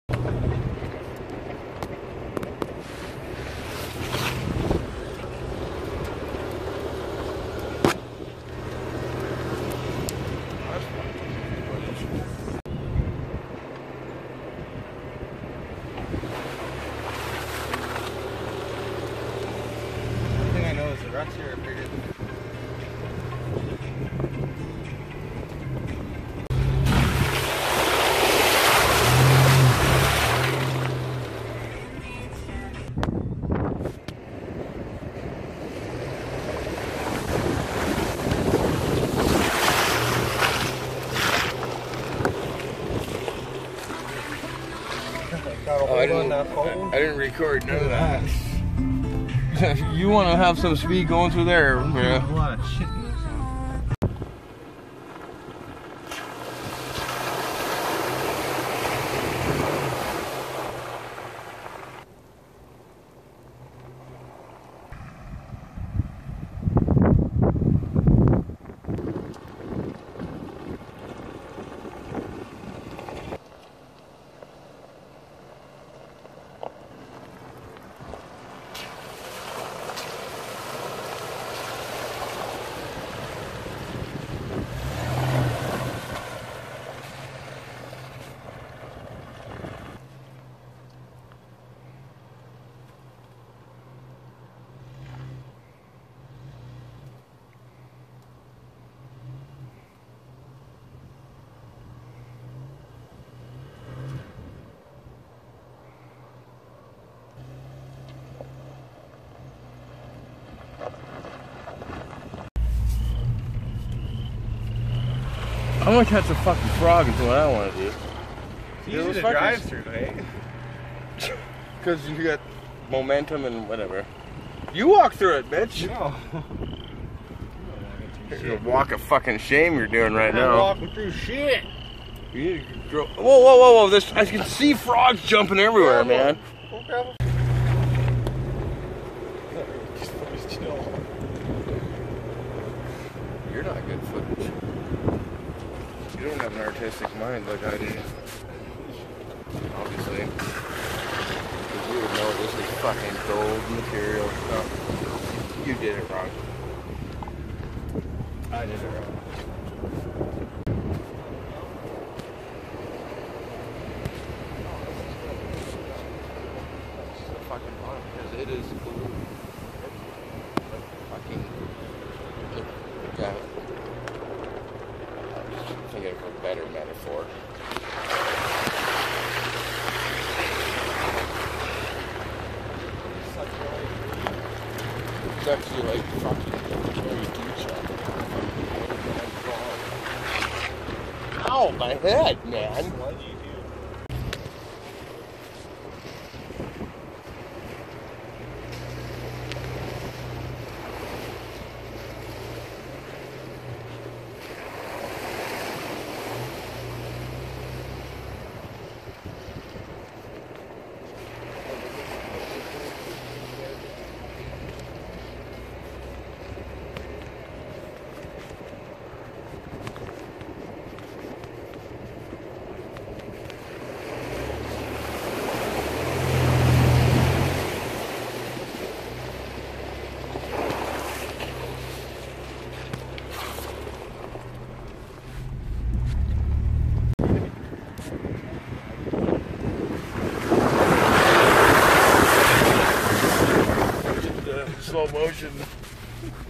In town, off road.
A little mud. July 21.19.. Sunday funday with the son.